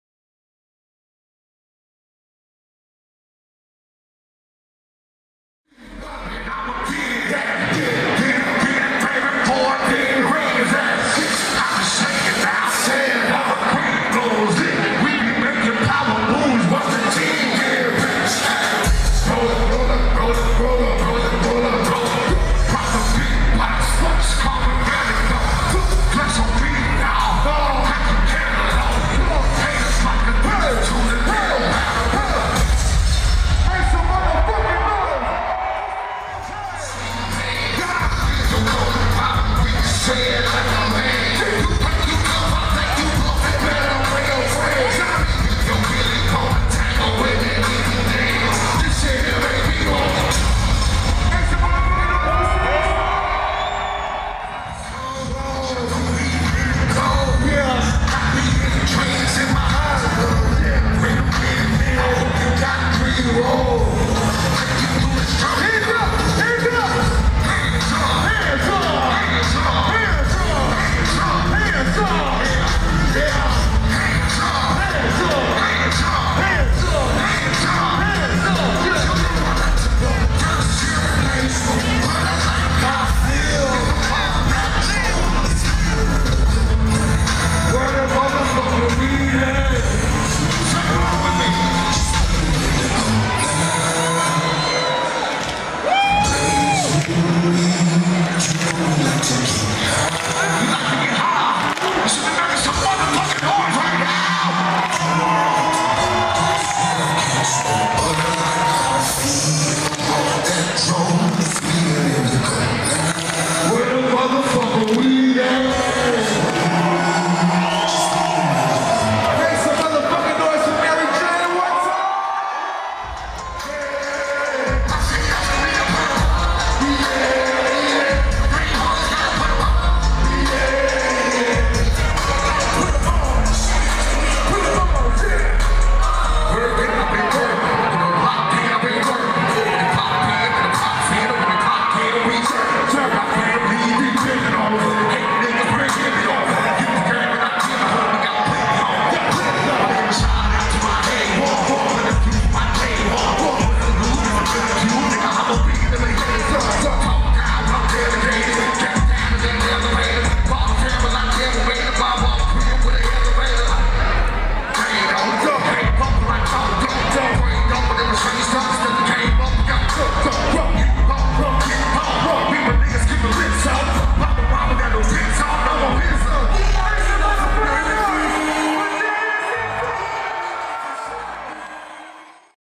Stevie Stone (Strictly Strange 2017 - Worcester, MA) 5/13/2017
Short clips of Stevie Stone performing live at the Worcester Palladium in Worcester, MA.

SUPPORT YOUR FAVORITE ARTISTS!